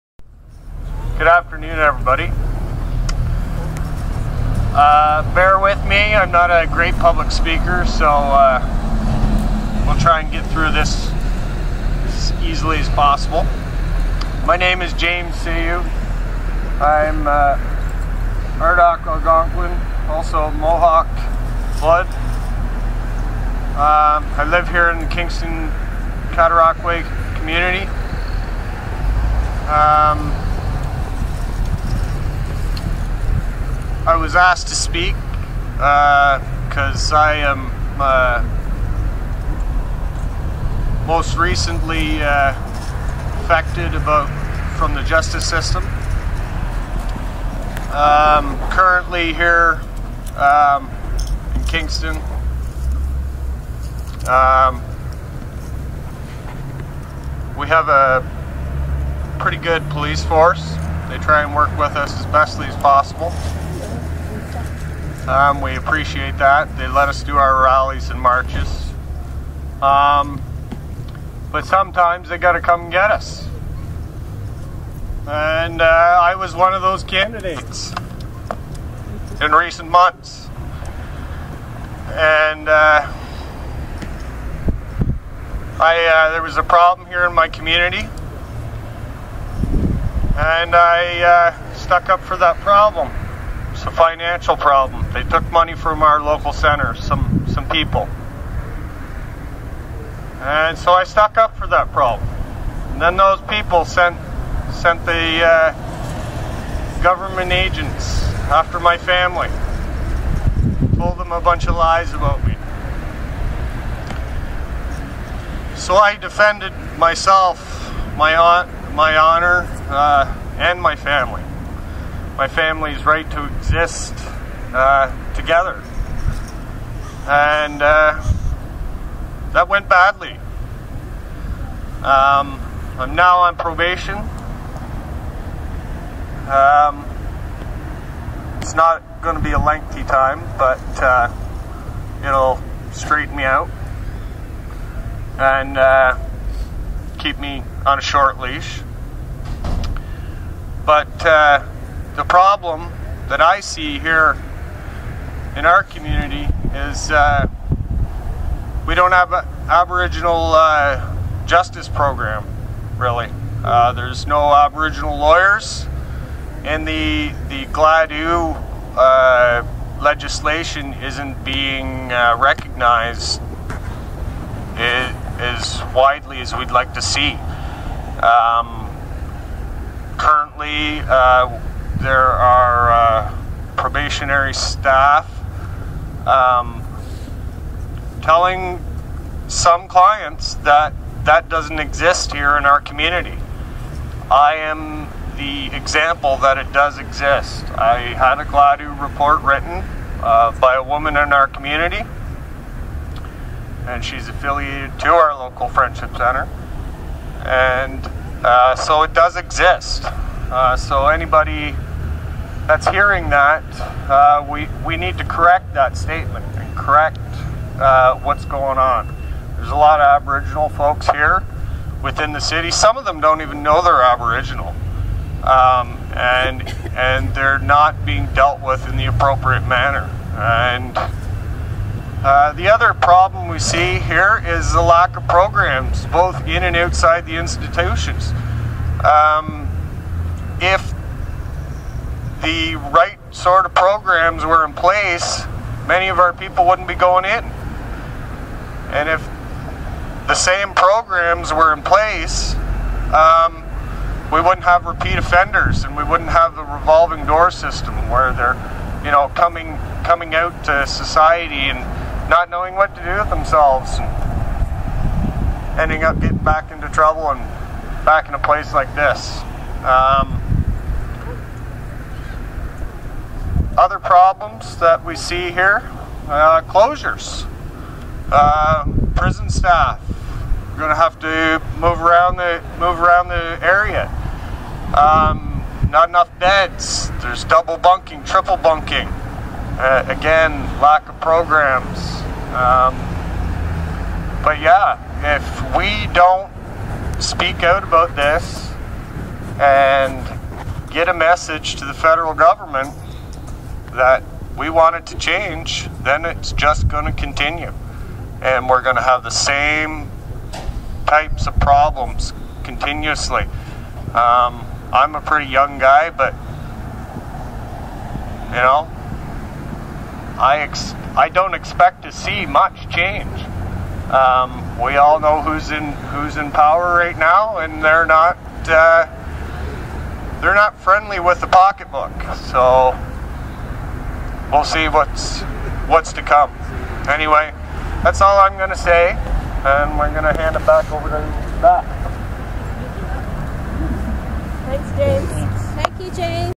National Day of Action - Indigenous Prisoners Justice: James Sayeau
Speech from James Sayeau, Idle No More, Kingston, Ontario.